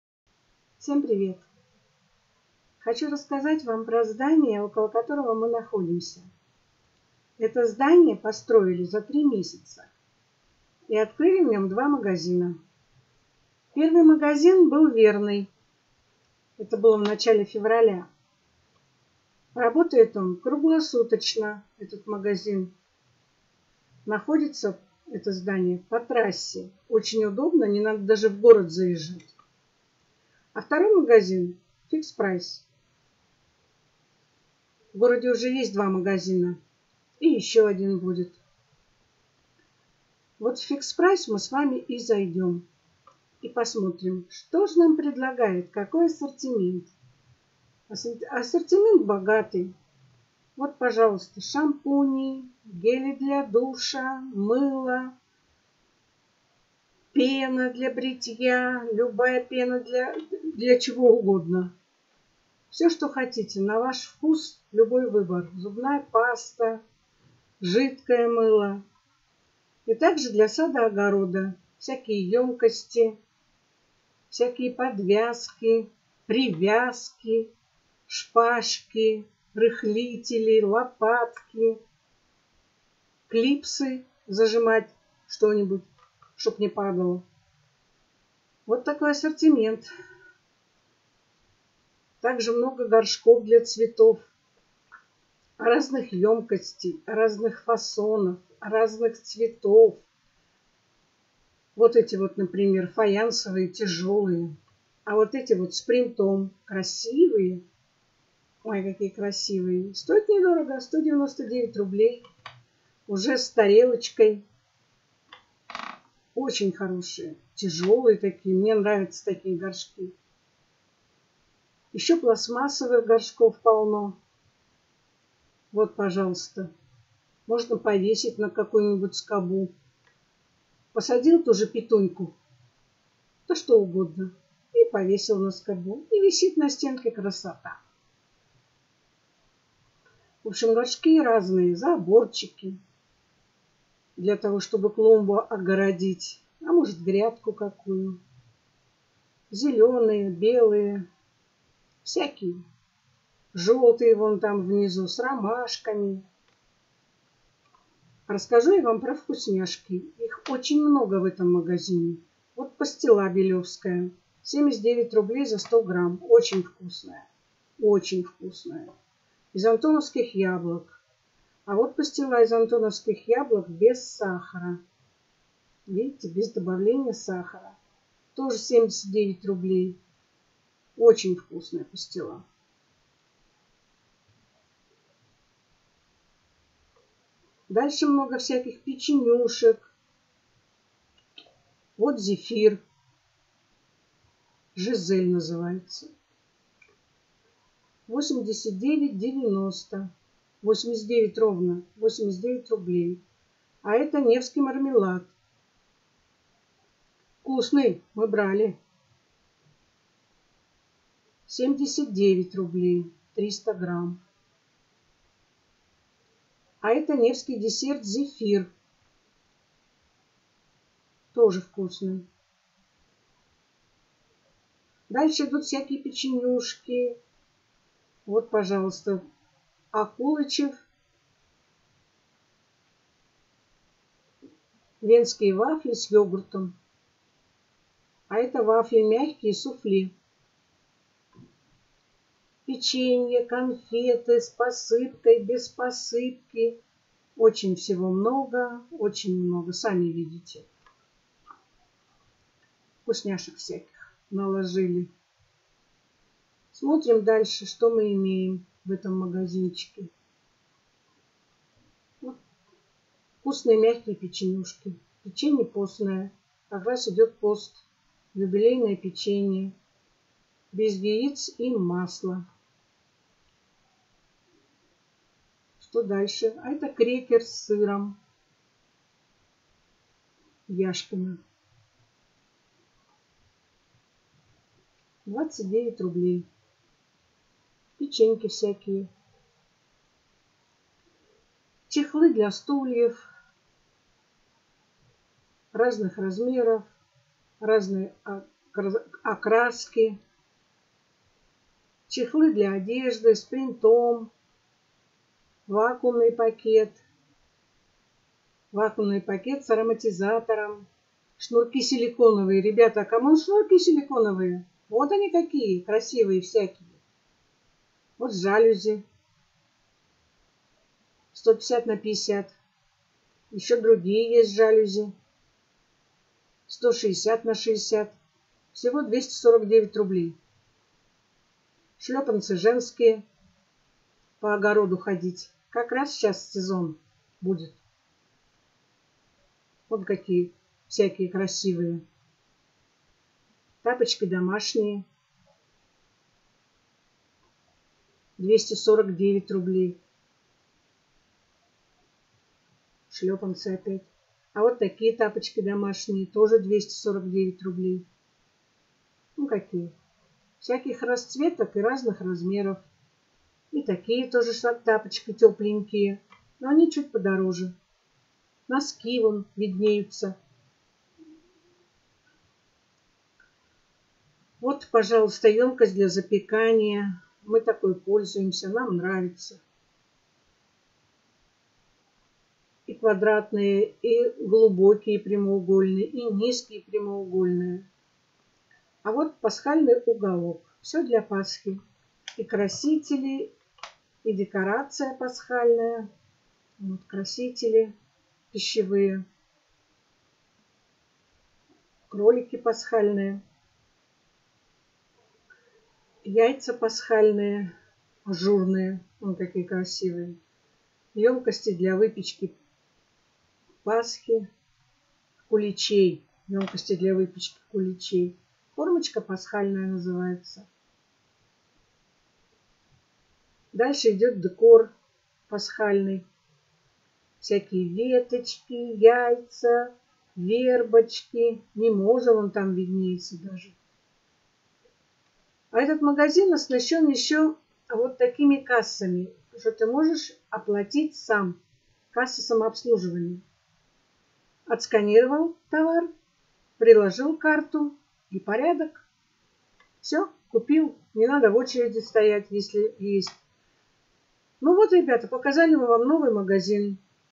Магазин ФИКС ПРАЙС при въезде в город
Открылся новый магазин Фикс Прайс. Магазин удачно расположен на трассе Кольчугино - Владимир, можно в город даже и не заезжать. Рядом универсам Верный, что также удобно, теперь можно сократить время при поездке по магазинам. Зашли посмотреть чем торгуют. Показываем вам.